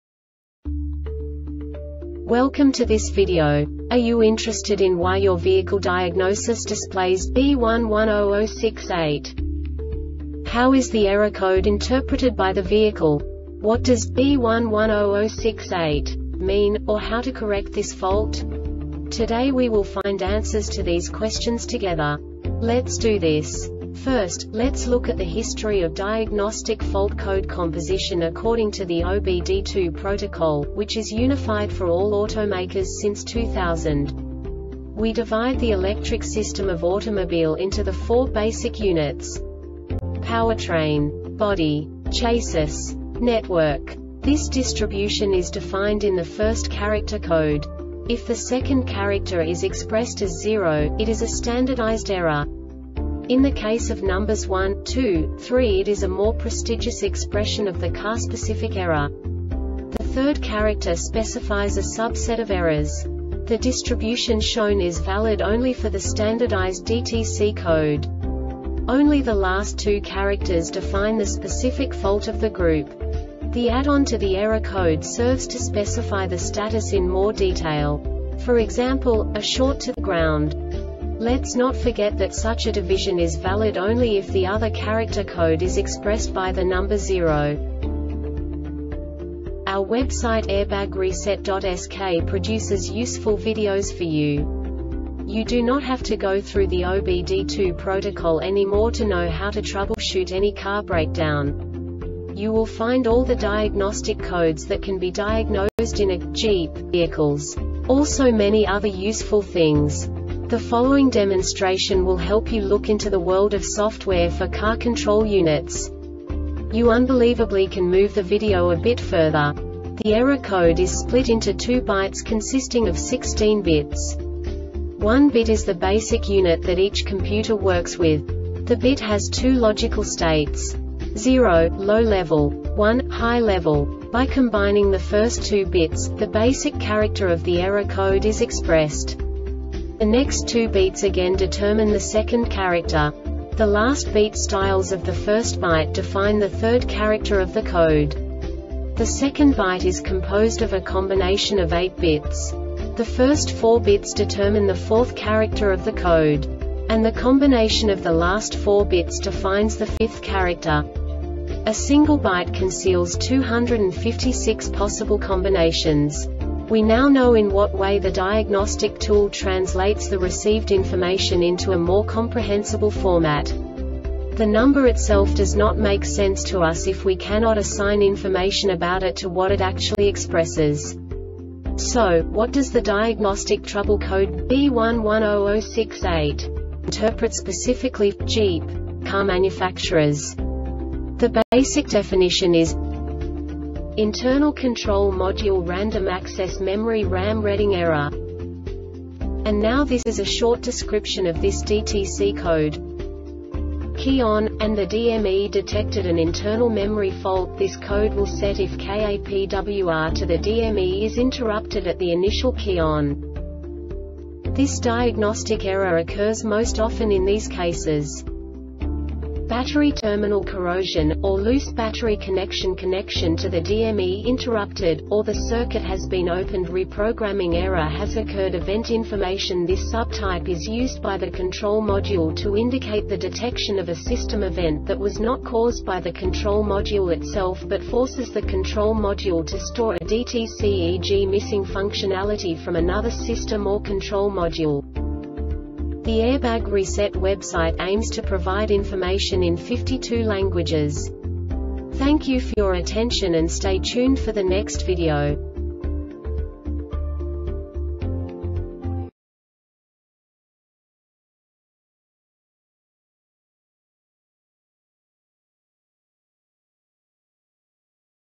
DTC Jeep B1100-68 Short Explanation
Contents:
0:21 Basic DTC analysis according to OBD2 protocol standard.
1:48 Insight into programming
2:58 A diagnostically understandable description of the Diagnostic Trouble Code
3:05 Basic error definition

Make:
Jeep
Code:
B1100 68
Definition:
Internal Control Module Random Access Memory (RAM) Reading Error
Description:
Key on,and the DME detected an internal memory fault.This code will set if KAPWR to the DME is interrupted (at the initial key on).
Cause:
Battery terminal corrosion,or loose battery connectionConnection to the DME interrupted,or the circuit has been openedReprogramming error has occurred
Failure Type:
Event Information